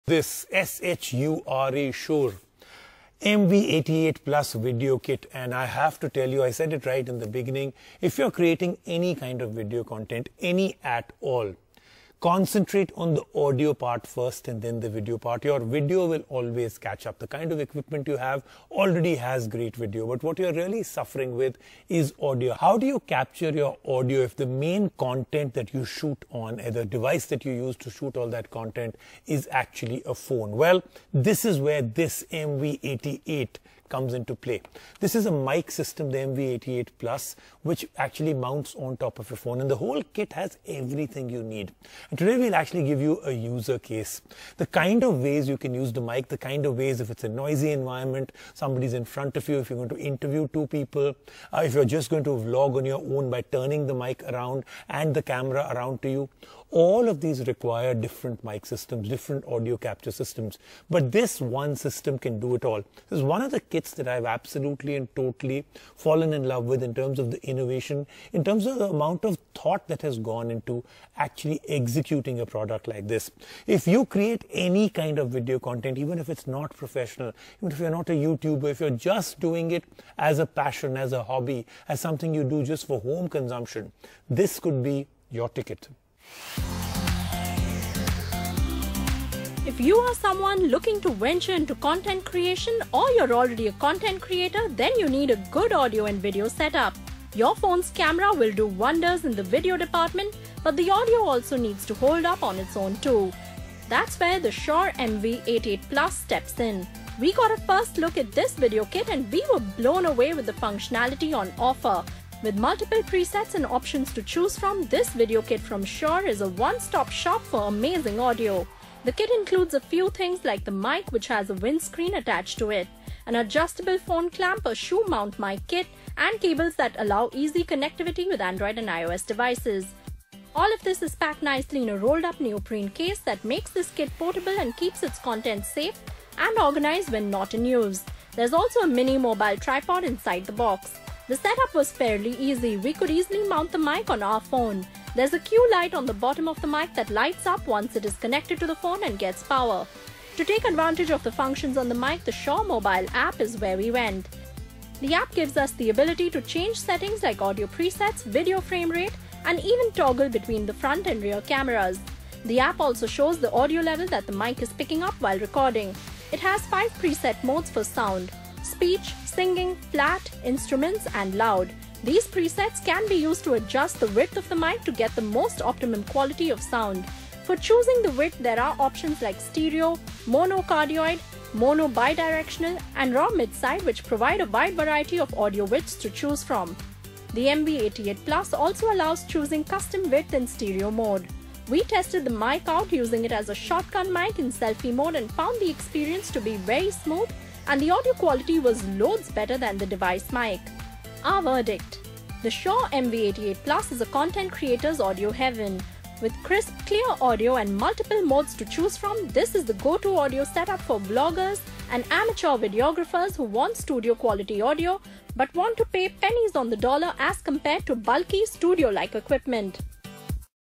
Audio Enhanced With Shure MV88+
The Shure MV88+ is a game-changer for video production as it enhances the audio capabilities of your smartphone. It provides studio-like quality at a great price point.